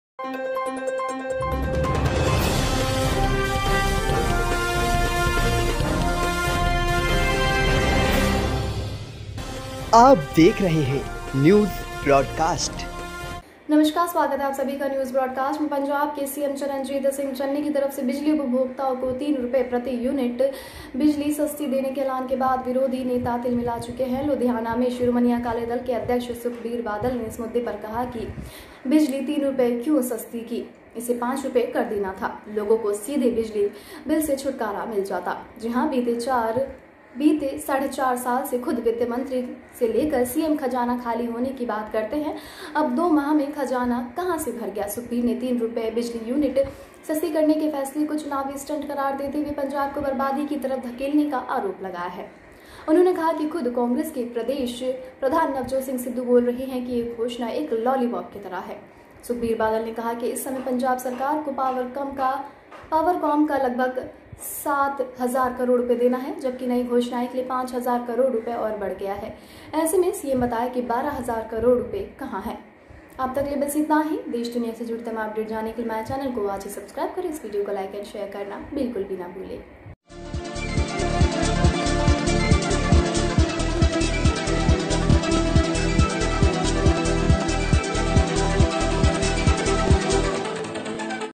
सुखबीर बादल ने चन्नी सरकार पर साधा निशाना: बिजली बिल सस्ती करने को लेकर कैसा तंज
पंजाब के मुख्यमंत्री चरणजीत सिंह चन्नी की तरफ से बिजली उपभोक्ताओं को तीन रुपये प्रति यूनिट बिजली सस्ती देने के एलान के बाद विरोधी नेता तिलमिला चुके हैं। लुधियाना में शिरोमणि अकाली दल के अध्यक्ष सुखबीर बादल ने इस मुद्दे पर कहा कि बिजली तीन रुपये क्यों सस्ती की, इसे पांच रुपये कर देना था, लोगों को सीधे बिजली बिल से छुटकारा मिल जाता।